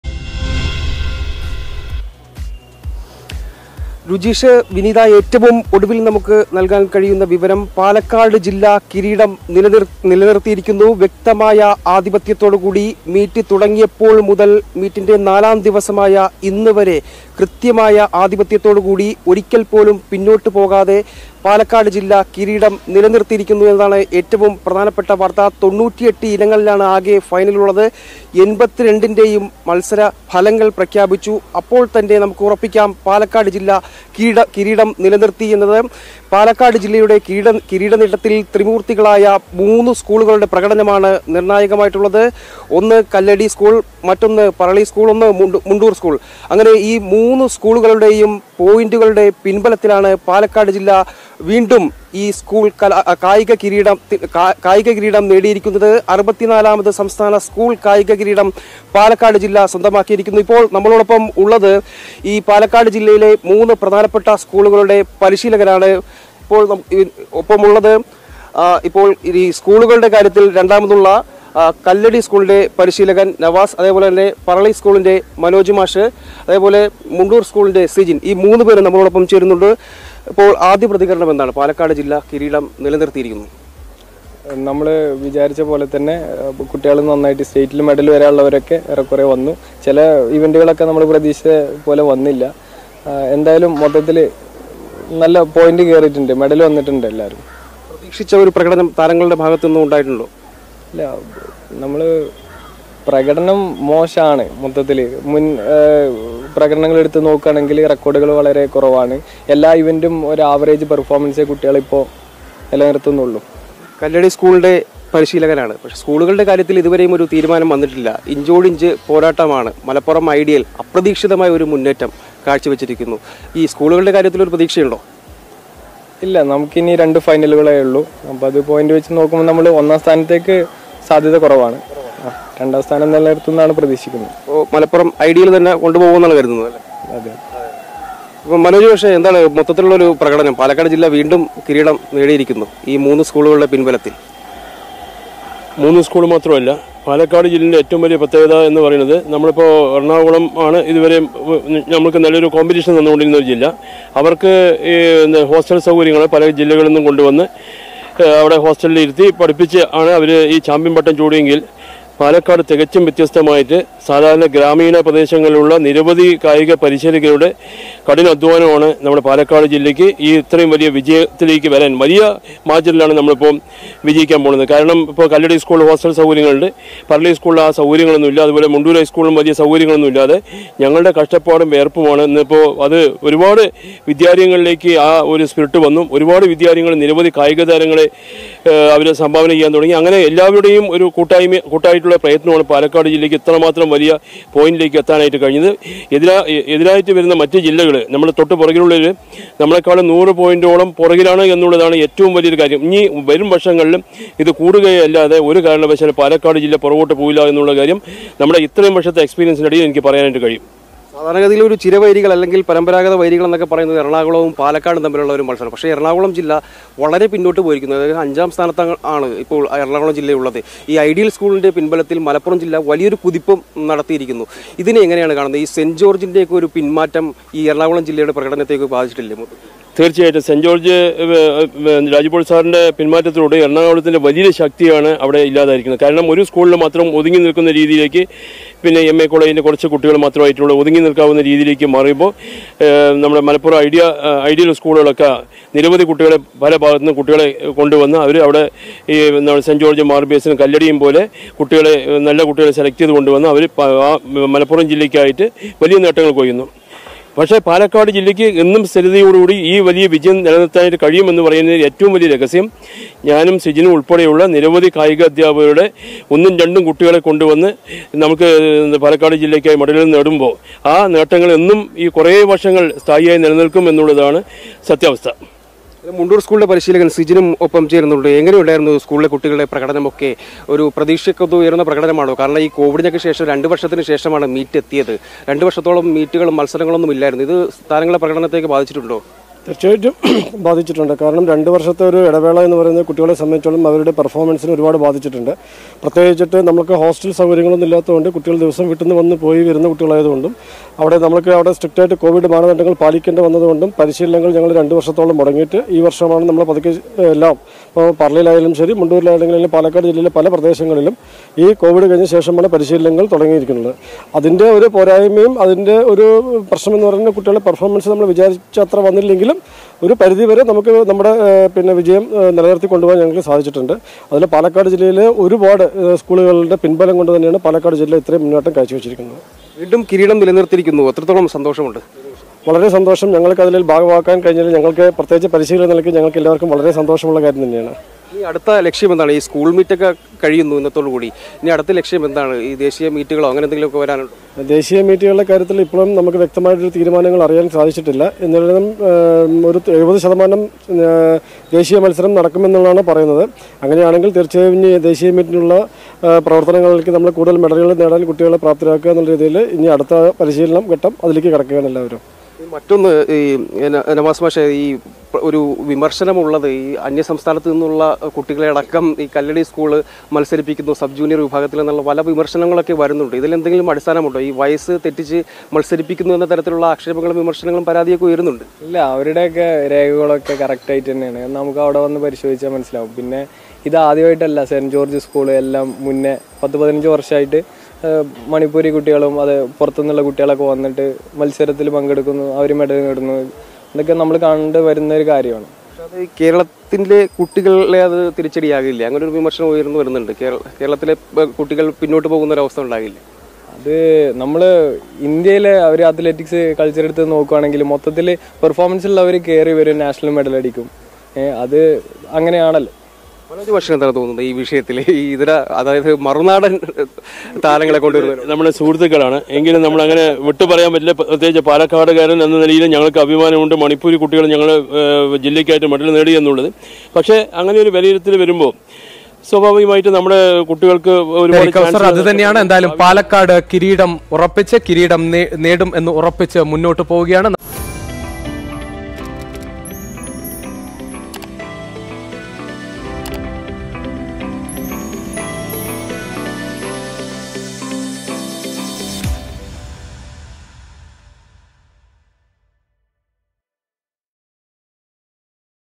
കായികോത്സവത്തിൽ പാലക്കാടൻ പടയോട്ടം; കോട്ട കാത്ത് പരിശീലകർ| Kerala School Sports Meet
കായികോത്സവത്തിൽ പാലക്കാടൻ പടയോട്ടം; കോട്ട കാത്ത് പരിശീലകർ, കുട്ടികളുടെ കഷ്ടപ്പാടും വിയർപ്പും ഫലം കണ്ടു, വരും വർഷങ്ങളിലും പാലക്കാട് മുന്നോട്ട് കുതിക്കുമെന്ന് പരിശീലകർ‌